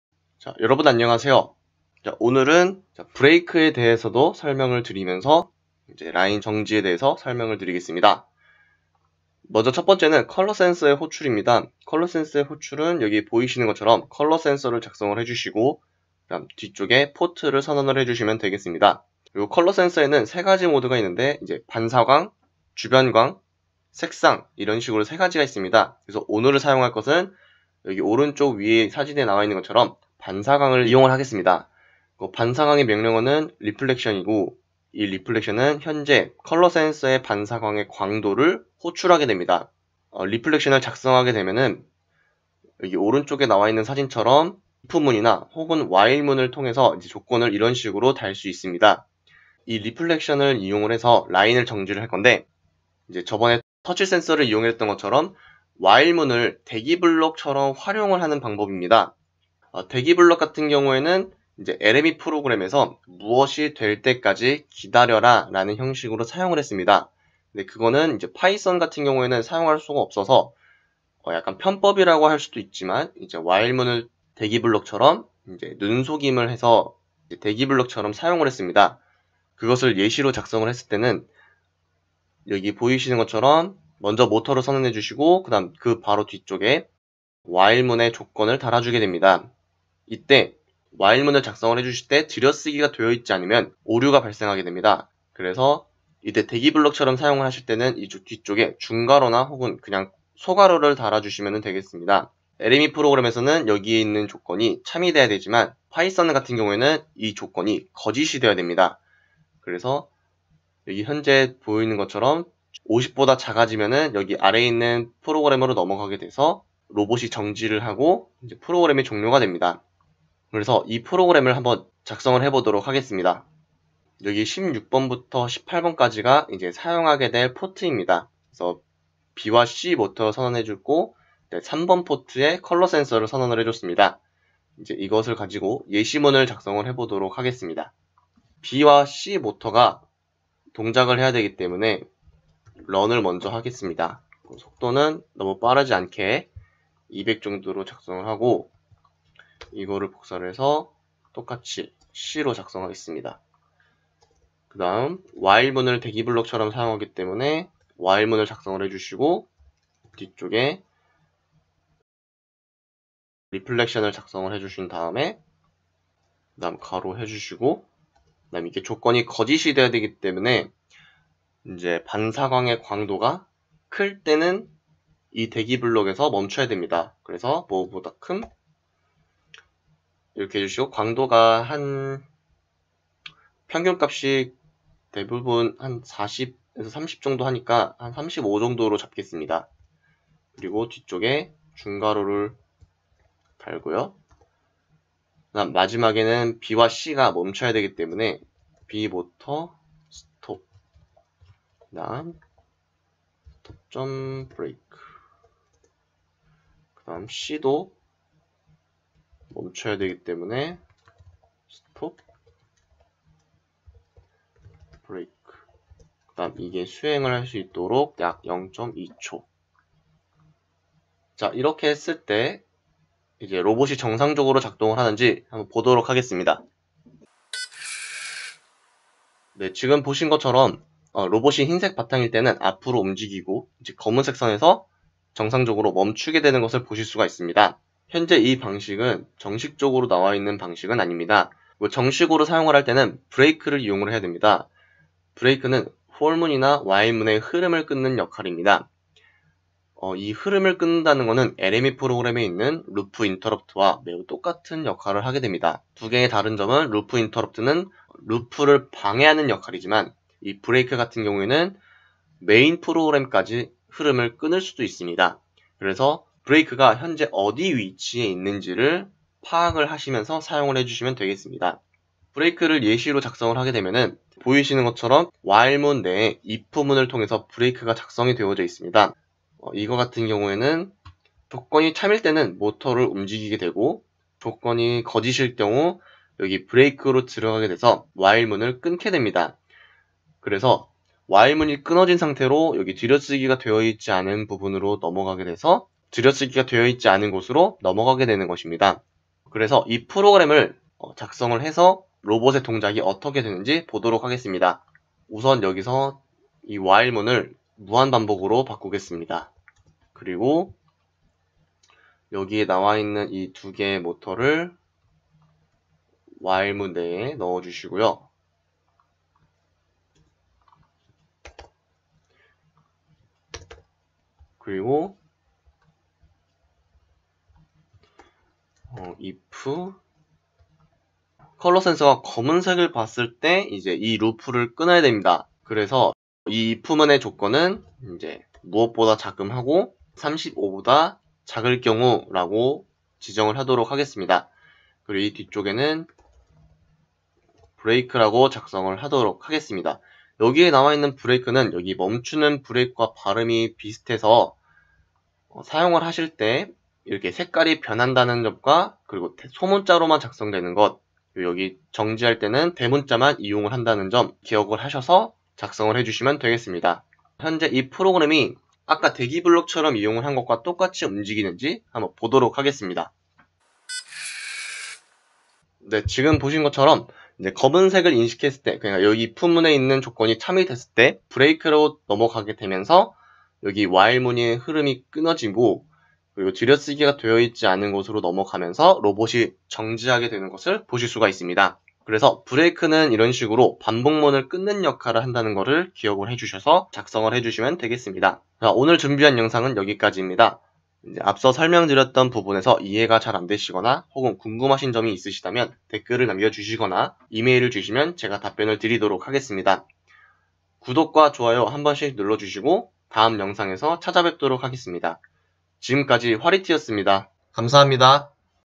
[EV3 Python] - 검은선에서 정지하기(Feat. break) - 화리트(Hwarite)
이번 영상은 Python을 통해서 검은 선에서 정지하는 방법에 대한 설명입니다. 첫번째 설명은 while문을 대기 블록 처럼 활용하는 방법에 대한 설명이고, 두번째는 for문과 while문의 흐름을 끊는 break 명령어에 대한 설명입니다. break는 LME 프로그램의 루프 인터럽트와 같은 역할이지만 main 프로그램까지 끊을 수 있어서 어디 위치에 있는지가 중요합니다.

00:09 컬러센서 선언
00:54 while문을 대기 블록 처럼 사용하기
02:03 프로그램 작성1
03:45 로봇 동작 예시1
04:04 break 설명
05:05 프로그램 작성2
06:12 로봇 동작 예시2